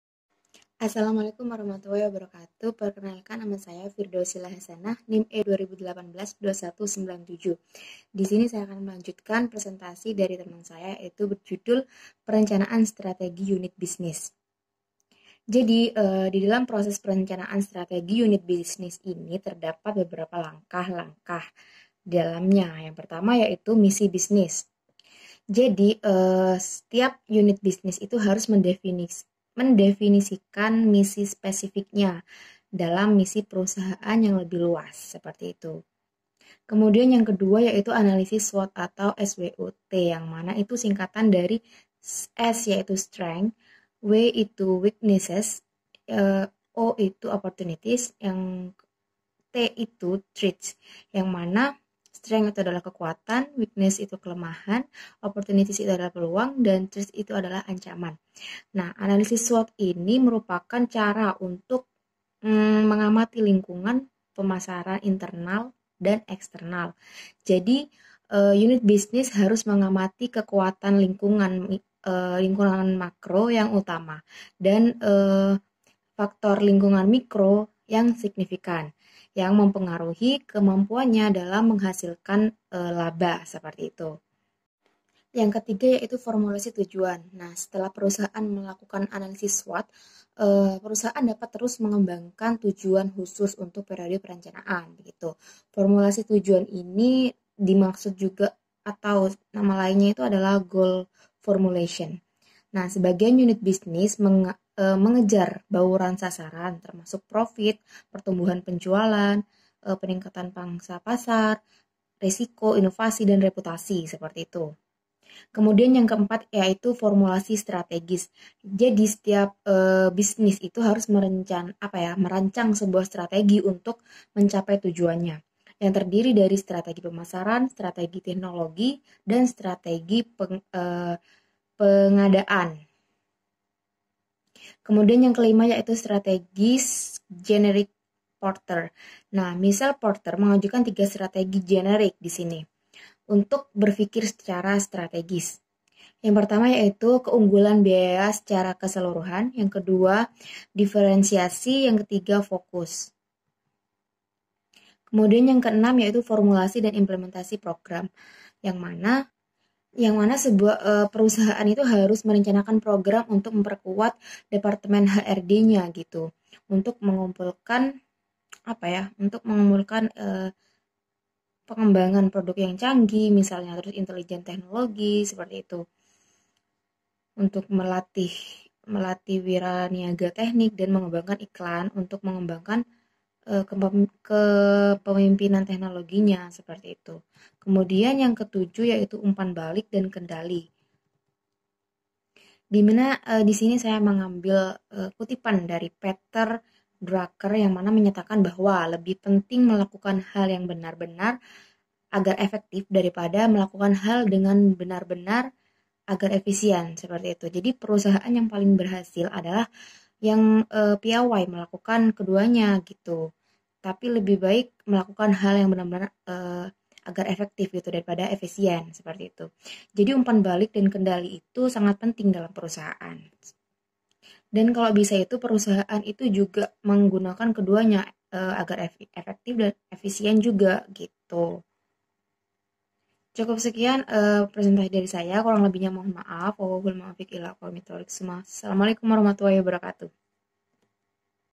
KELOMPOK 2/ES 4/Part 3/Firdausila Hasana/E20182197/Perencanaan strategi unit bisnis
Perencanaan Strategi Unit Bisnis

Proses perencanaan strategi unit bisnis terdiri dari langkah-langkah sebagai berikut:
1. Misi bisnis.
Setiap unit bisnis harus mendefinisikan misi spesifiknya didalam misi perusahaan yang lebih luas.
2. Analisis SWOT
Keseluruhan evaluasi tentang kekutan, kelemahan, peluang dan ancaman disebut analisis SWOT. Analisis SWOT  merupakan cara untuk mengamati lingkungan pemasaran internal dan eksternal.
3. Formulasi Tujuan
Setelah perusahaan melakukan analisis SWOT, perusahaan dapat terus mengembangkan tujuan khusus untuk periode perencanaan.
4. Formulasi Strategis
Setiap bisnis harus merancang sebuah strategi untuk mencapai tujuannya, yang tediri dari strategi pemasaran serta strategi teknologi dan strategi pengadaan yang kompatibel.
5. Strategis Generik Porter
6. Formulasi dan Implementasi Program
Strategi pemasaran yang bagus dapat gagal akibat implementasi yang buruk. Jika sebuah perusahaan memutuskan untuk mempertahankan kepemimpinan perusahaannya, perusahaan itu harus merencanakan program untuk memperkuat departemen HRD-nya.
7. Umpan Balik dan Kendali
Penyesuaian strategis perusahaan dengan lingkungan akan sangat terganggu, karena lingkungan pasar berubah lebih cepat daripada tujuh S perusahaan (strategi - struktur - sistem -style - skill - staf - shared value).